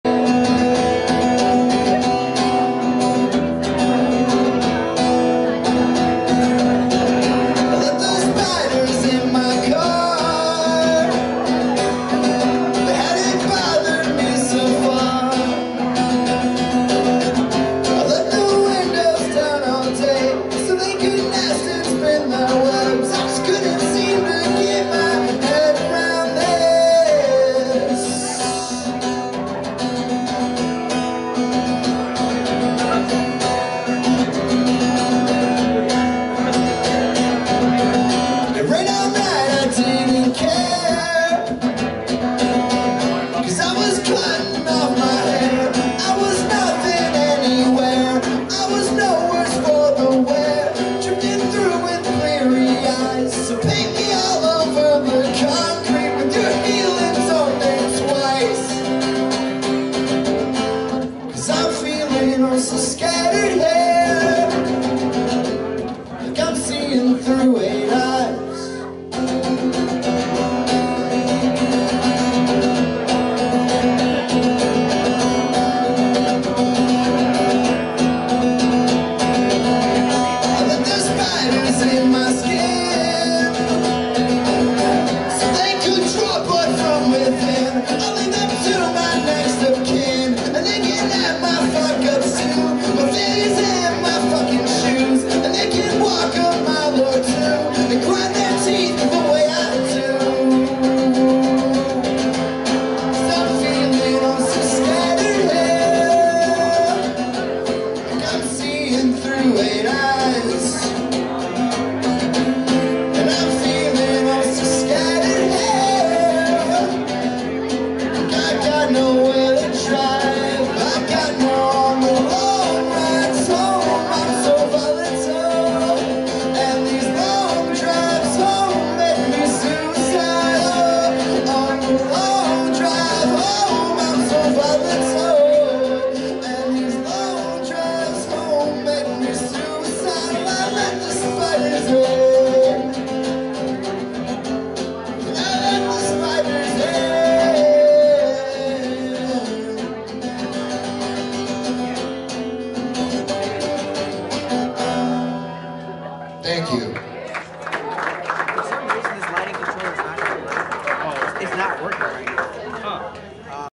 4/9: House of Wills plays at Grog Shop Cleveland Ohio
House of Wills plays at Grog Shop Cleveland Ohio on 20150920

About House of Wills:
Josh Voland from ULTRA ULTRA and whoever else wants to be involved playing some songs straight from the heart. Better believe it.